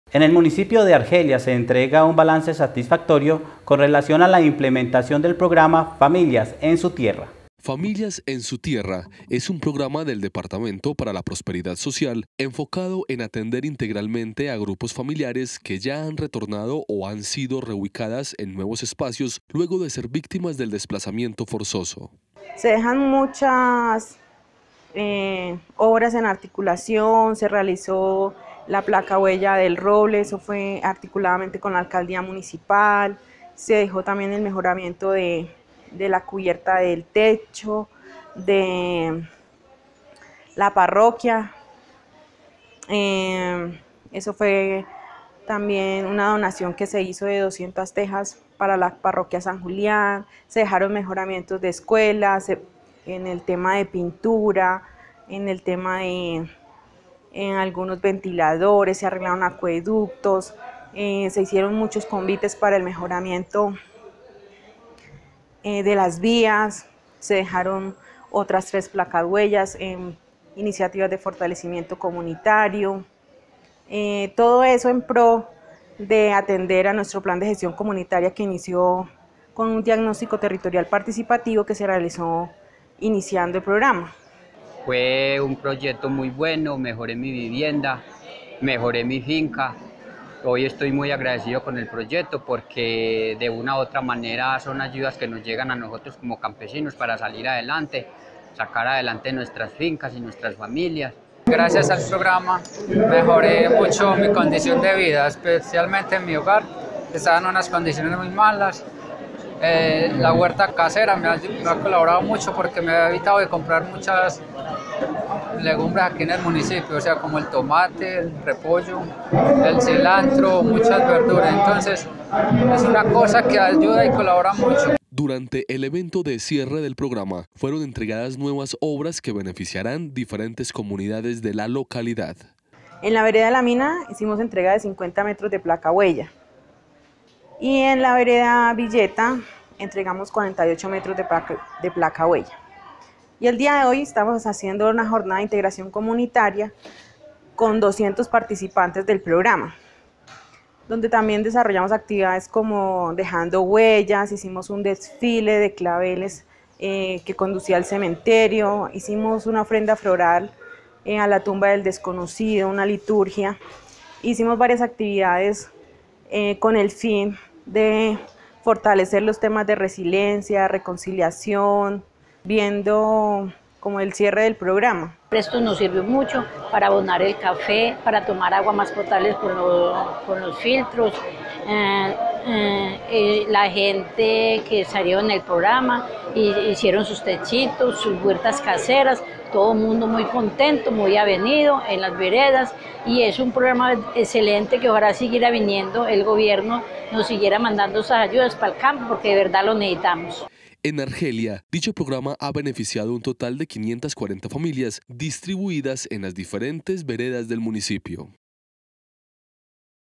CIERRE DEL PROGRAMA FAMILIAS EN SU TIERRA EN ARGELIA
En el municipio de Argelia se entrega un balance satisfactorio con relación a la implementación del programa “Familias en su Tierra”